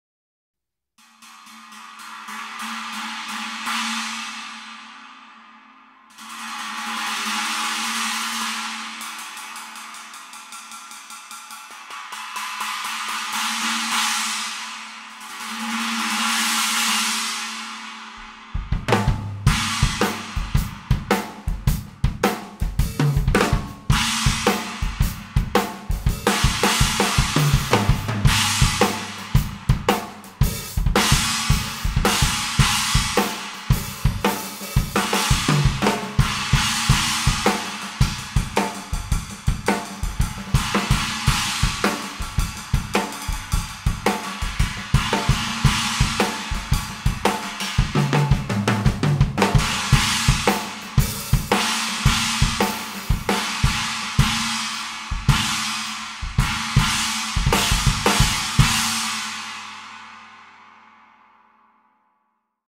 Zildjian 20" FX Oriental China Trash Cymbal - 1660g (A0620-1102521L)
Zildjian 20" FX Oriental China Trash Cymbal available from myCymbal.com

Other gear used in this video:
• Cymbals (From drummer's L to R): Zildjian 15" K Light Hi-Hats, Zildjian 20" K Crash Ride Cymbal